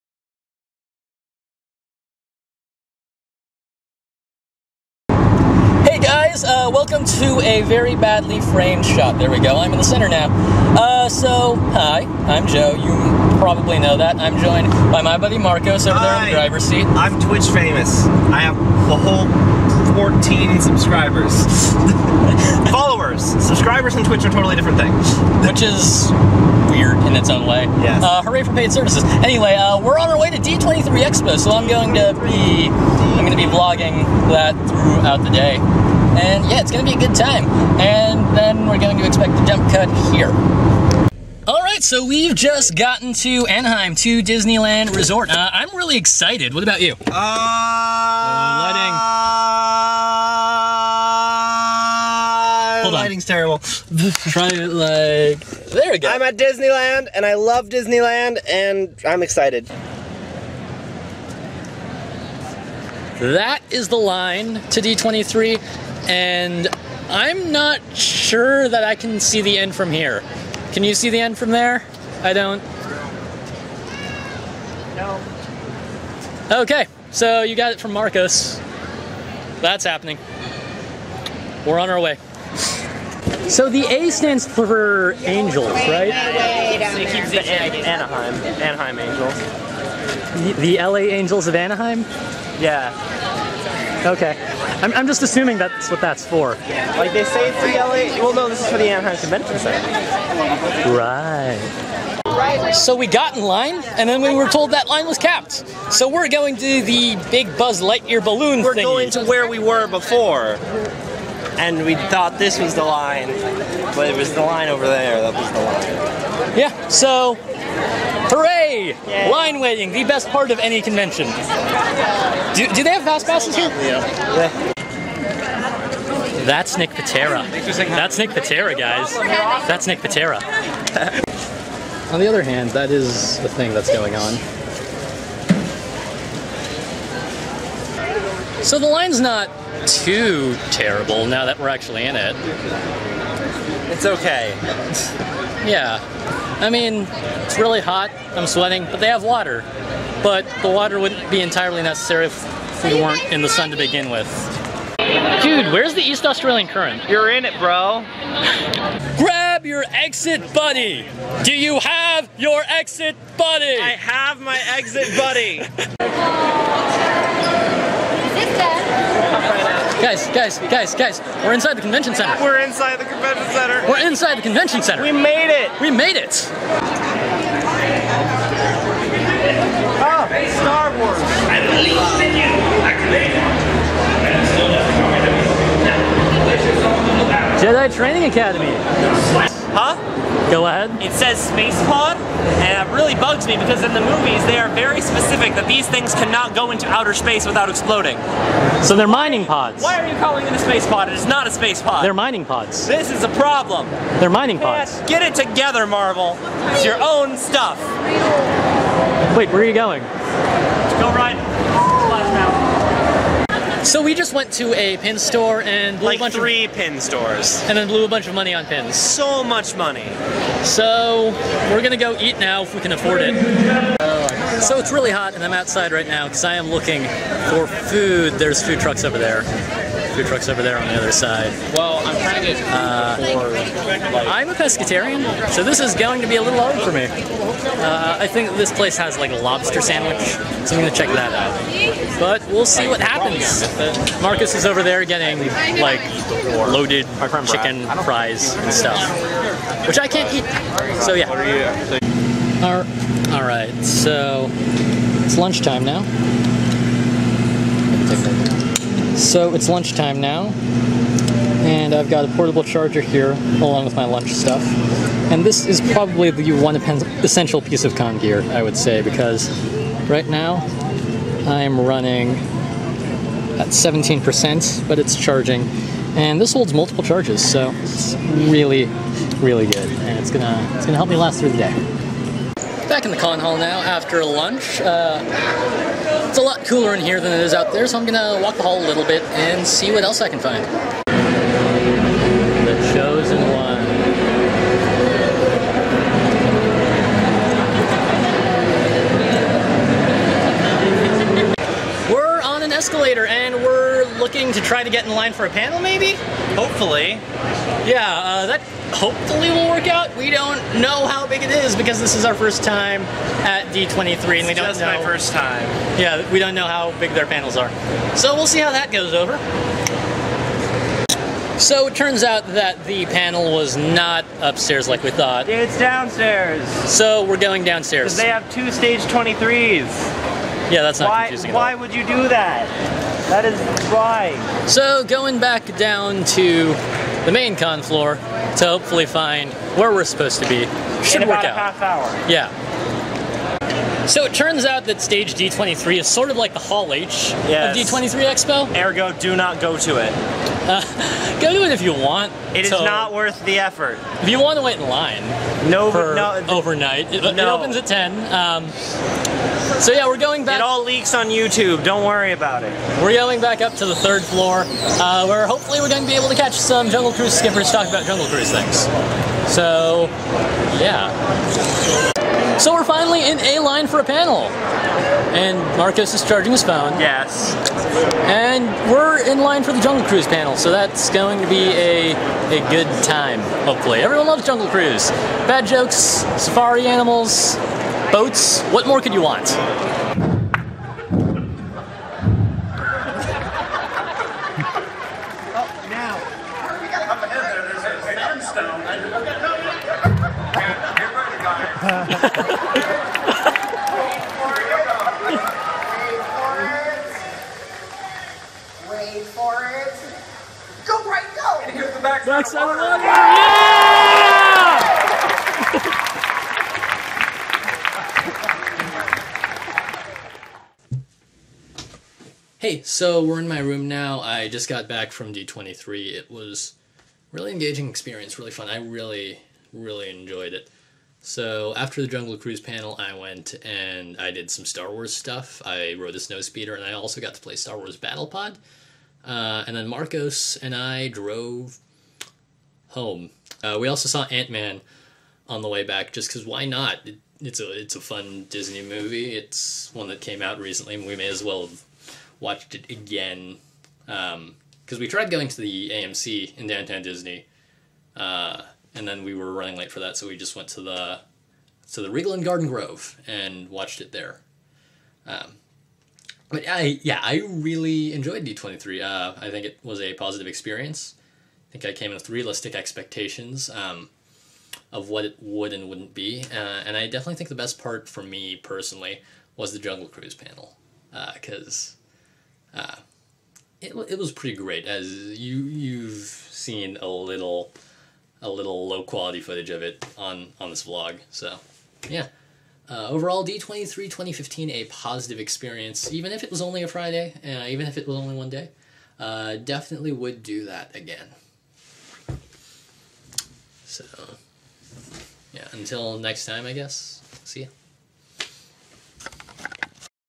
Vlogging with Joe (and Marcos): Joe and Marcos Go to D23 Expo 2015
Join Joe and Marcos as they wait in line at D23 Expo for the Expo itself, some Star Wars stuff, and the World Famous Jungle Cruise!